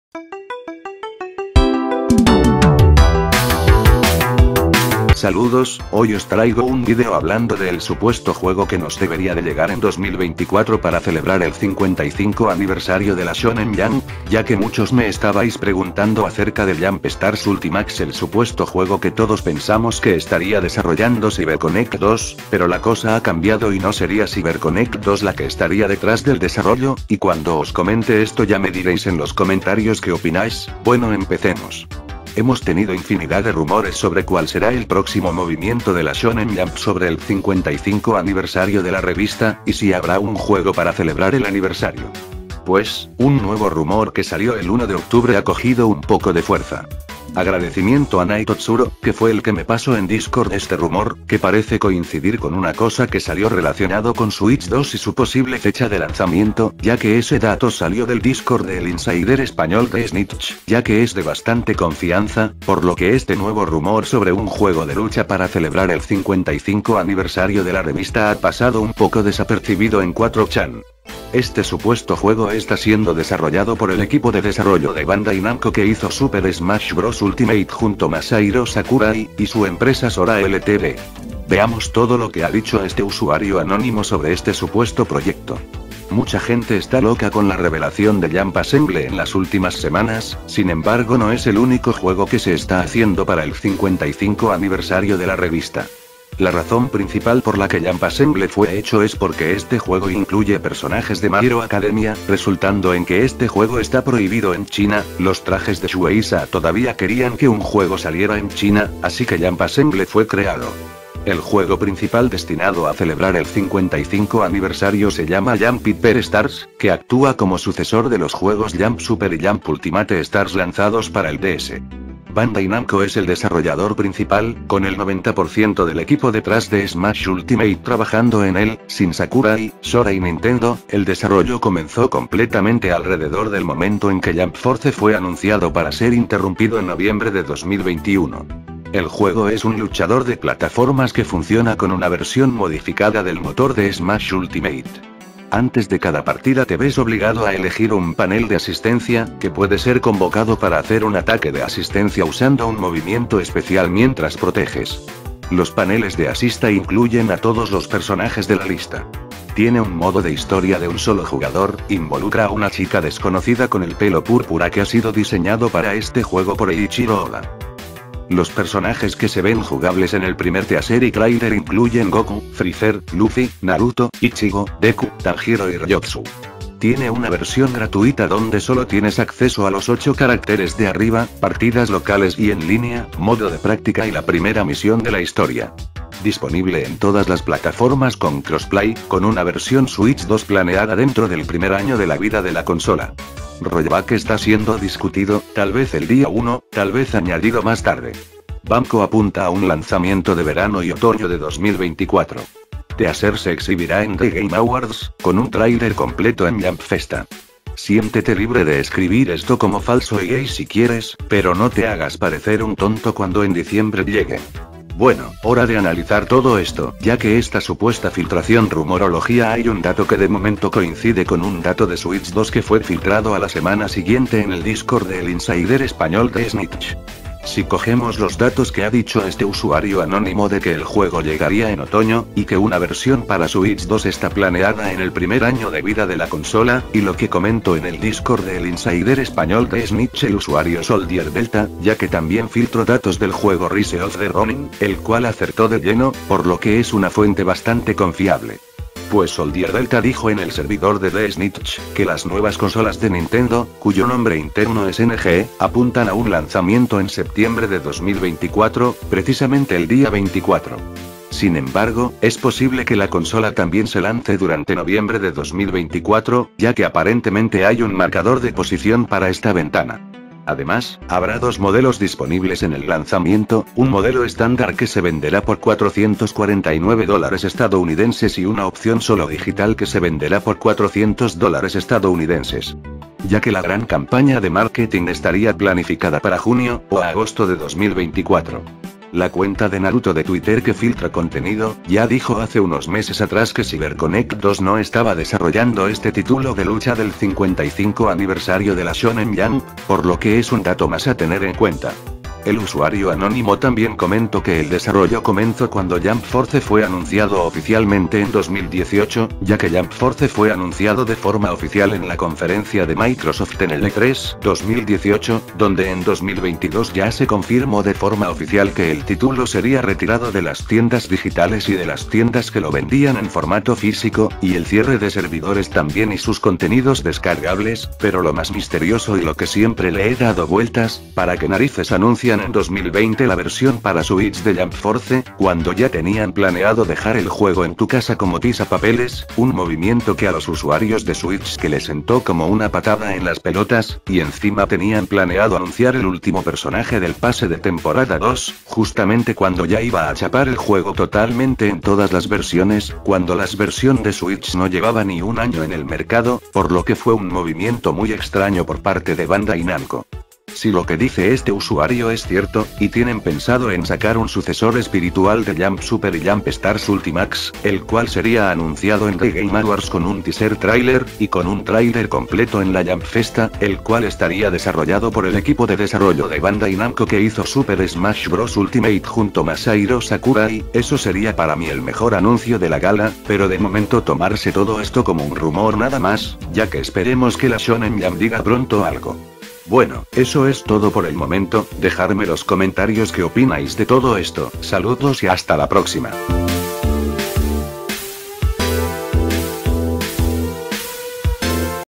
Rumor: ¿ Jump Hyper Stars ? ¿ Anuncio En The Game Awards ?
Este ha sido el ultimo Rumor entorno al proximo juego de lucha del 55 aniversario de la shonen jump.